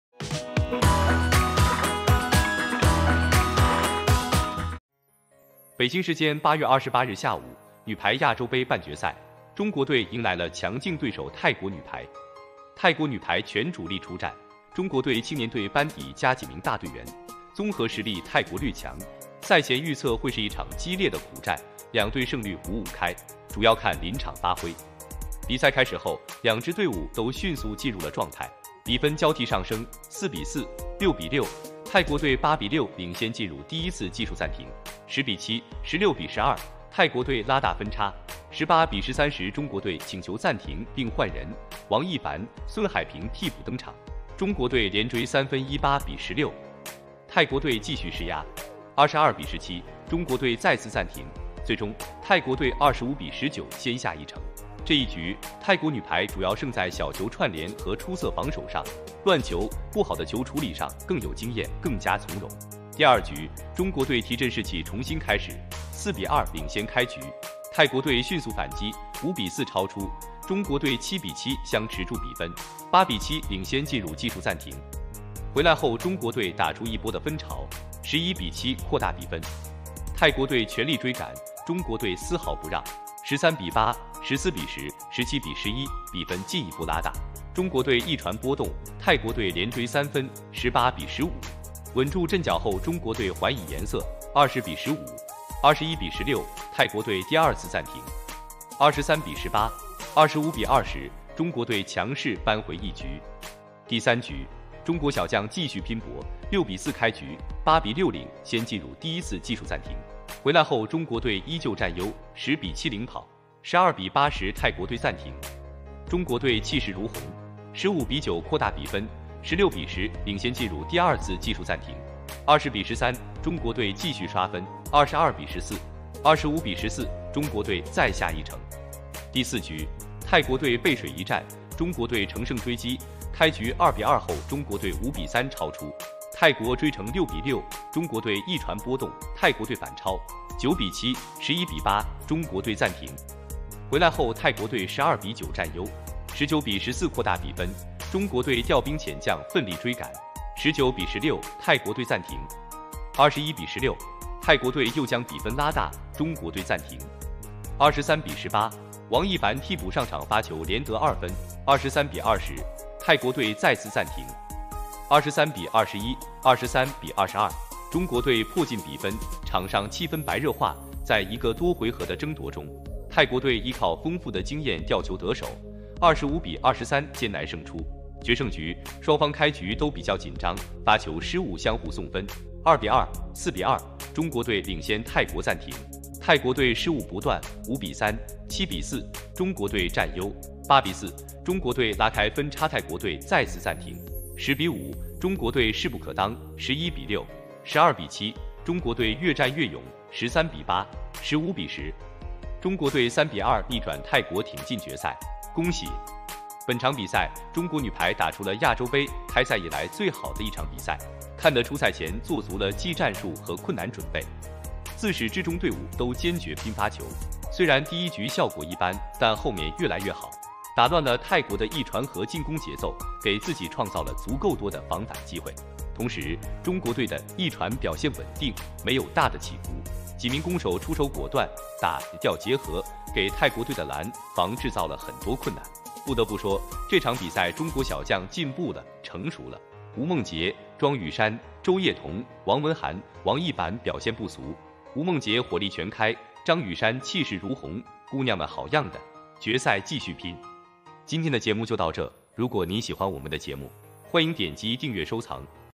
I吳夢潔火力全開，莊宇珊氣勢如虹，中國女排逆轉泰國挺進決賽
北京時間8月28日下午，女排亞洲杯半決賽，中國隊迎來了強勁對手泰國女排。泰國女排全主力出戰，中國隊青年隊班底加幾名大隊員，綜合實力泰國略強，賽前預測會是一場激烈的苦戰，兩隊勝率五五開，主要看臨場發揮。